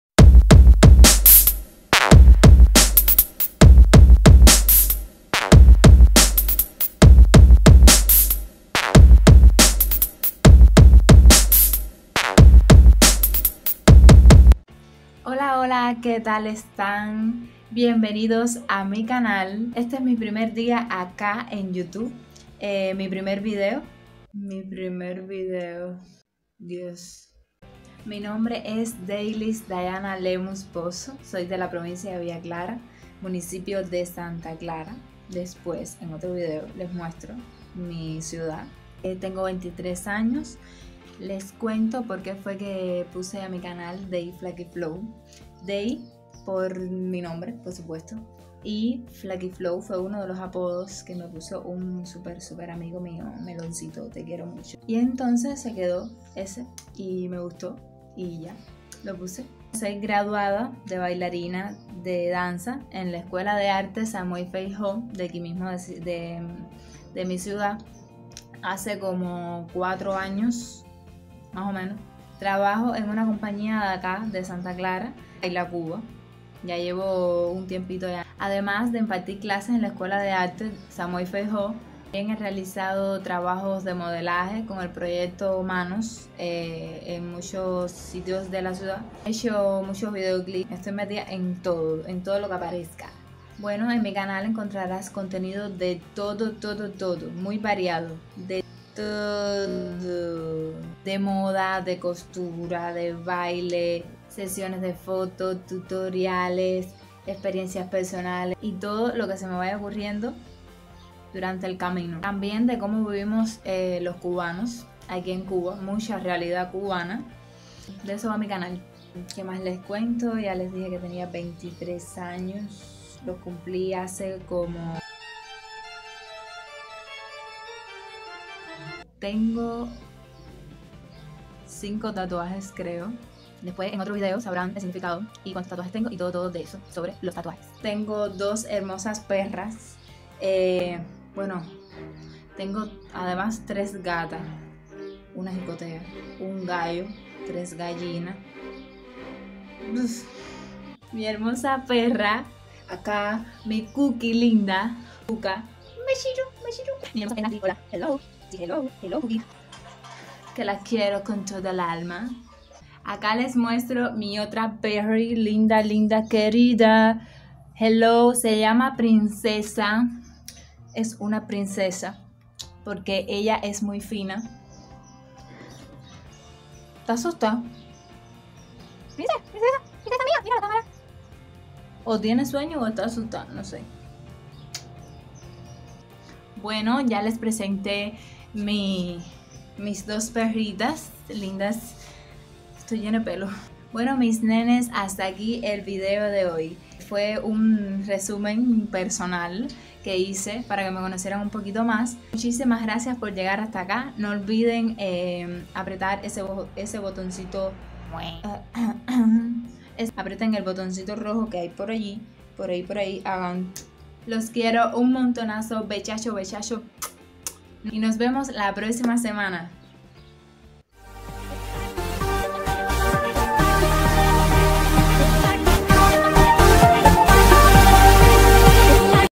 Mi primer video en YouTube 🇨🇺/@Dei Flaki Flow
Este es mi primervideo en YouTube,dónde me presento ,y doy alguna informaciónpersonal. Les cuento un poco de mi.